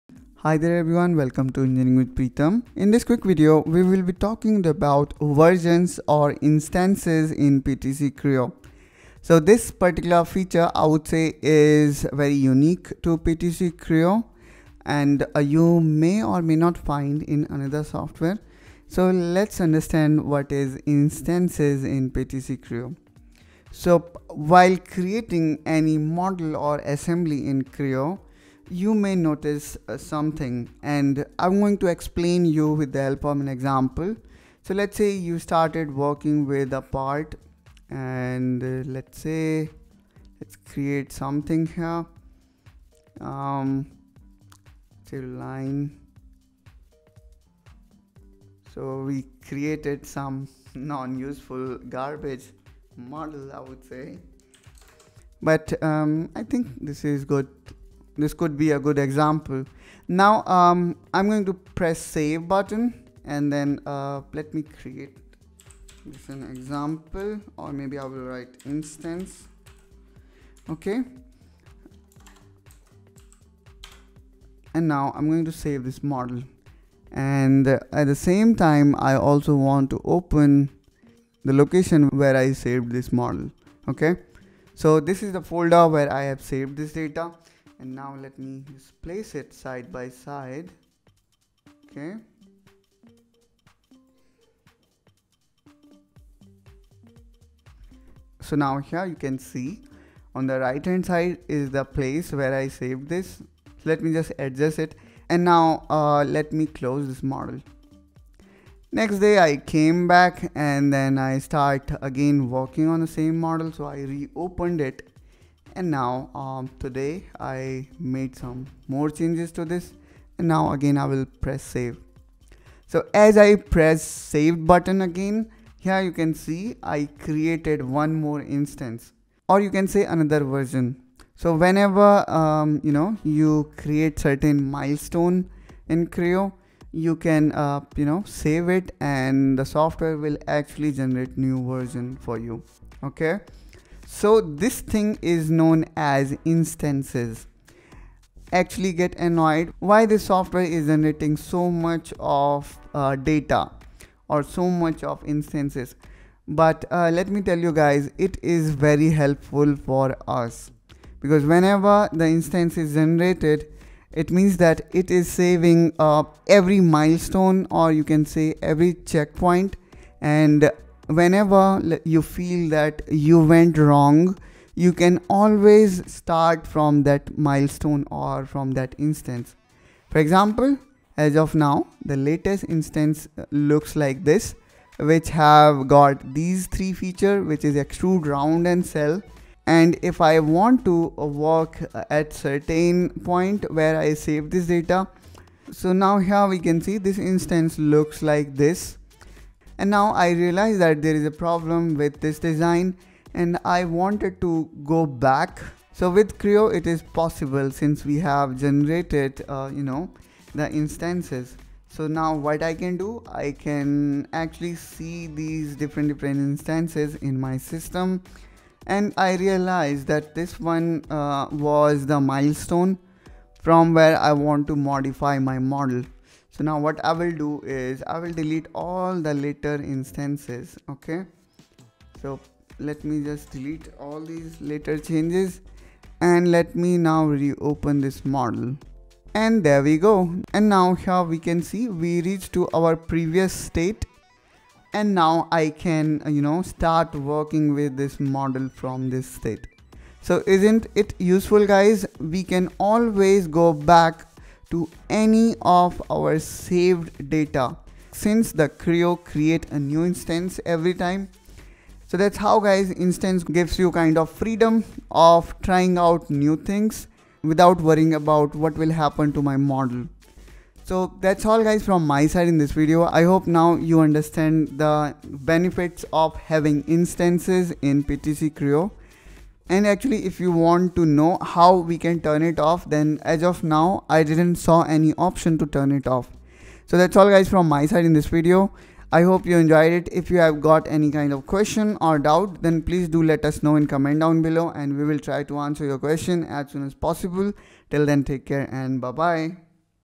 Instances in PTC Creo Parametric
Instances in PTC Creo
When we save 3d model or any file in PTC Creo, The Creo always generate new version. This is known as instance in creo in this video we will talk about instance and how it is beneficial for us.